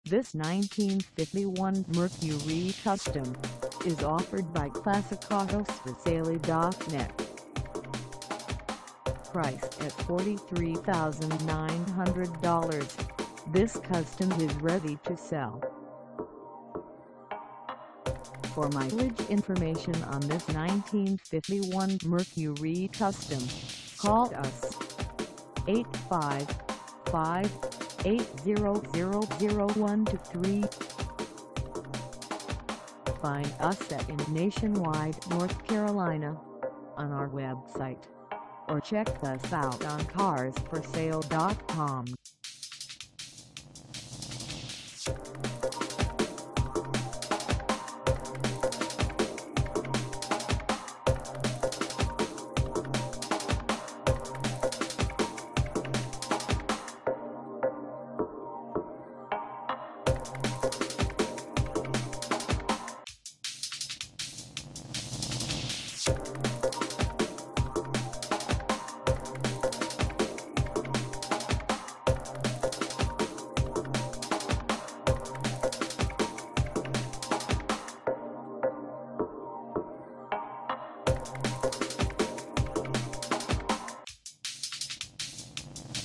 1951 Mercury Custom for sale in Nationwide, NC 27603 at Cla
This 1951 Mercury Custom  is for sale in Nationwide, NC 27603 at Classicautosforsale.net.
 392090
Engine: Unspecified 350
Transmission: Automatic
Color: Unspecified
Mileage: 0
Contact Classicautosforsale.net Nationwide, NC 27603 to test drive this 1951 Mercury Custom today. List your classic . The best classic car advertising on the planet!